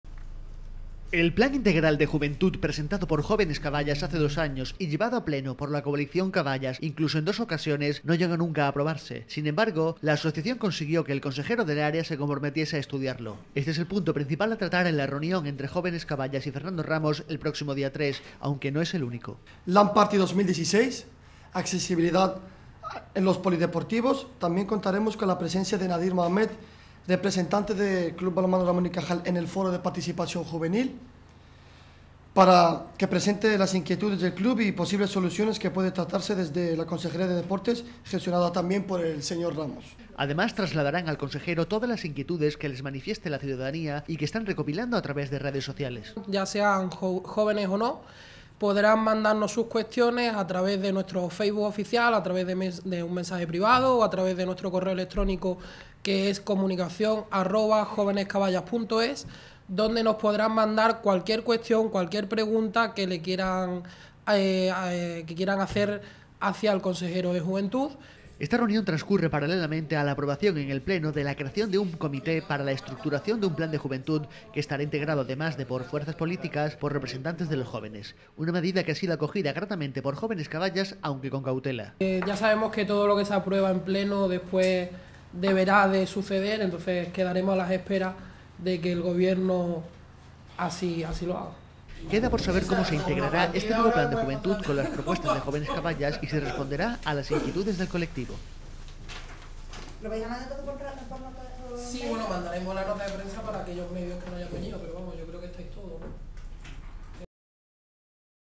Jóvenes Caballas se reunirán con Fernando Ramos para abortar el plan de Juventud
El Consejero de Juventud, Deporte, Turismo y Fiestas recibirá durante la tarde del próximo jueves, 3 de marzo a los representantes de Jóvenes Caballas para dialogar sobre el plan integral de juventud que ya presentaron hace dos año y que al  iniciar la nueva legislatura Fernando Ramos prometió estudiar. Aprovecharan la reunión para plantearle otras cuestiones como la LAN party 2016, y trasladarle las preguntas de la ciudadanía que están recopilando a través de diferentes medios.